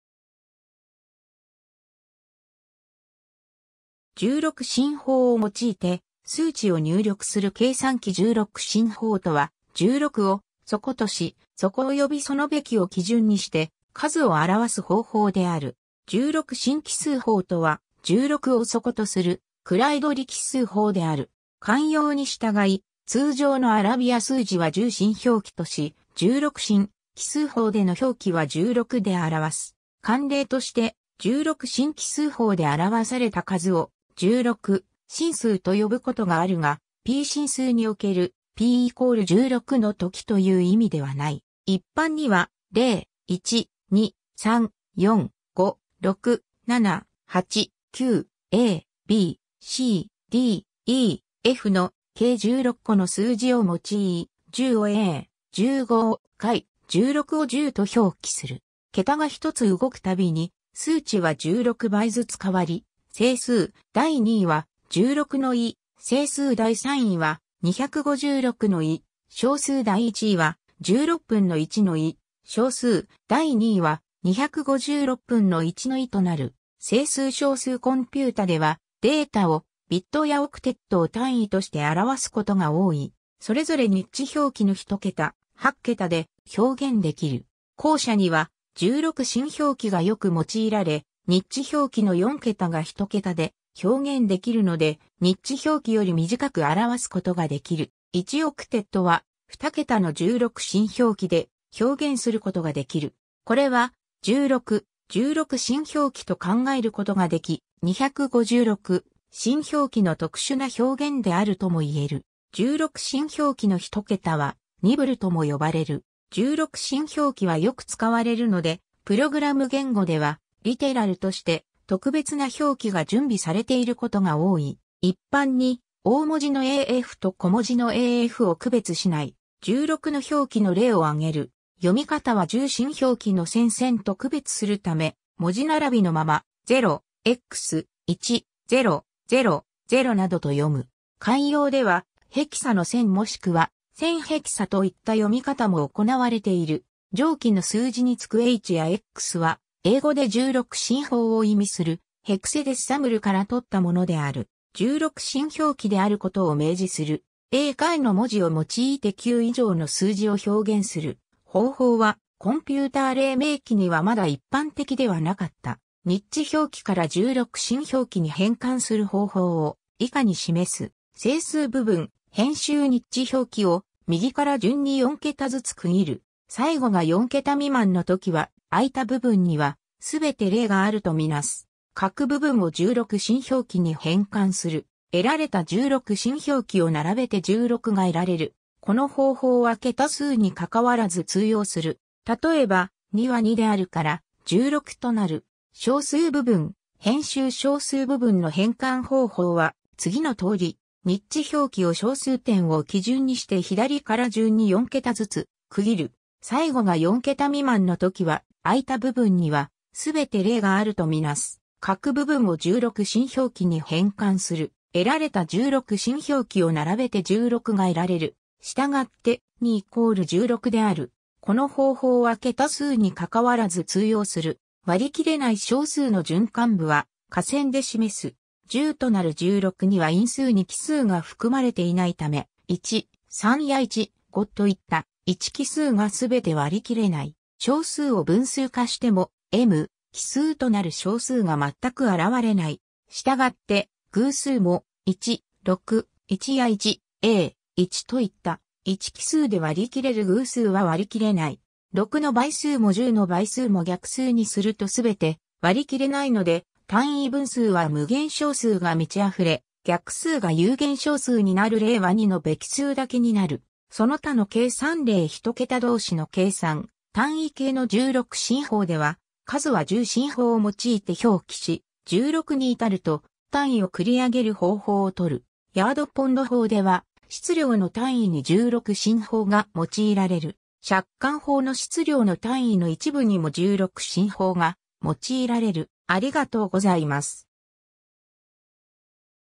十六進法
十六進法を用いて数値を入力する計算機 十六進法（じゅうろくしんほう、 英: hexadecimal）とは、16を底（てい）とし、底およびその冪を基準にして数を表す方法である。
 十六進記数法とは、十六を底とする位取り記数法である。
慣用に従い、通常のアラビア数字は十進表記とし、十六進記数法での表記は ( )16（括弧および下付きの16）で表す。
慣例として、十六進記数法で表された数を「十六進数」と呼ぶことがあるが、「p進数における p = 16 のとき」という意味ではない。
桁が一つ動く度に数値は 16 倍ずつ変わり、整数第二位は「十六の位」、整数第三位は「二百五十六の位」、小数第一位は「十六分の一の位」、小数第二位は「二百五十六分の一の位」となる。
 整数 小数 コンピュータでは、データをビットやオクテットを単位として表すことが多い。
それぞれ二進表記の1桁、8桁で表現できる。
後者には、十六進表記がよく用いられ、二進表記の4桁が1桁で表現できるので、二進表記より短く表すことができる。
1オクテットは、2桁の十六進表記 (0x00 - 0xff) で表現することができる。
これは、十六・十六進表記と考えることができ、二百五十六進表記の特殊な表現であるともいえる。
 十六進表記の1桁はニブルとも呼ばれる。
 十六進表記はよく使われるので、プログラム言語ではリテラルとして特別な表記が準備されていることが多い。
一般に、大文字の A-F と小文字の a-f を区別しない。
 (1000)16 の表記の例を挙げる。
 読み方は十進表記の1000（（いっ）せん）と区別するため、文字並びのまま「ぜろ・エックス・いち・ぜろ・ぜろ・ぜろ」などと読む。
慣用では「ヘキサの千」もしくは「千ヘキサ」と言った読み方も行われている。
 上記の数字に付く h や x は英語で十六進法を意味する hexadecimal から取ったものである。
十六進表記であることを明示する。
 A - F の文字を用いて 9 以上の数字を表現する方法はコンピューター黎明期にはまだ一般的ではなかった。
 二進表記から十六進表記に変換する方法を、以下に示す。
 整数部分[編集] 二進表記を右から順に4桁ずつ区切る。
最後（最左部分）が4桁未満のときは、空いた部分（左側）には全て0があるとみなす。
 各部分を十六進表記に変換する。
 得られた十六進表記を並べて (3A)16 が得られる。
 この方法は桁数に関わらず通用する。
例えば、(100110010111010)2 は (0100, 1100, 1011, 1010)2 であるから、(4CBA)16 となる。
 小数部分[編集] 小数部分の変換方法は、次のとおり。
 二進表記を小数点を基準にして左から順に4桁ずつ区切る。
最後（最右部分）が4桁未満のときは、空いた部分（右側）には全て0があるとみなす。
 得られた十六進表記を並べて (0.D4)16 が得られる。
 したがって、(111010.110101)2 = (3A.D4)16 である。
 割り切れない小数の循環部は下線で示す。
「10」となる十六には因数に奇数が含まれていないため、1/3や1/5といった「1÷奇数」が全て割り切れない。
小数を分数化しても、「m/奇数」となる小数が全く現れない。
従って、偶数も、1/6｛1÷(2×3)｝や1/A｛1÷(2×5)｝といった「1÷奇数で割り切れる偶数」は割り切れない。
六の倍数も十の倍数も逆数にすると全て割り切れないので、単位分数は無限小数が充ち溢れ、逆数が有限小数になる例は2の冪数だけになる。
 その他の計算例 一桁同士の計算: 単位系の十六進法では、数は十進法を用いて表記し、16に至ると単位を繰り上げる方法を採る。
 ヤード・ポンド法では、質量の単位に十六進法が用いられる。
 尺貫法の質量の単位の一部にも十六進法が用いられる。